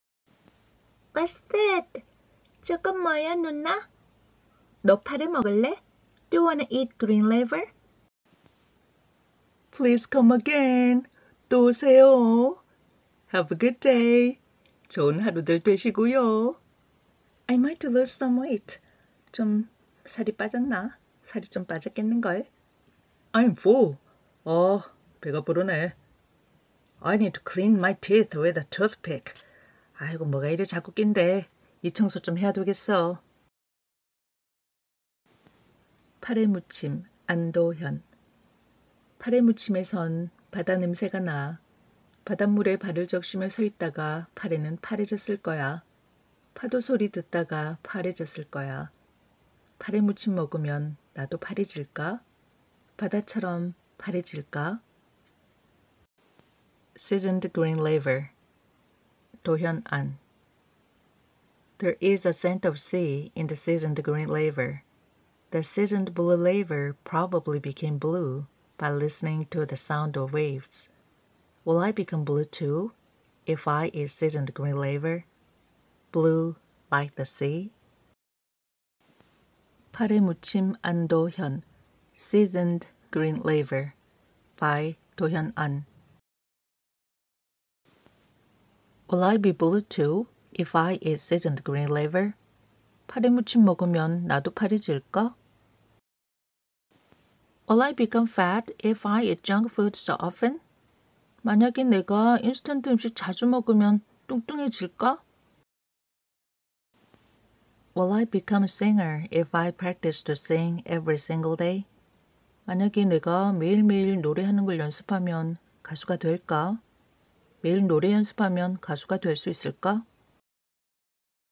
[번역동시] KoreanKidsPoem 파래무침 안도현동시 비룡소 엘리샤번역 | Korean/English
안도현시인의 동시.파래무침.영역후 필수 패턴으로 영어회화익히기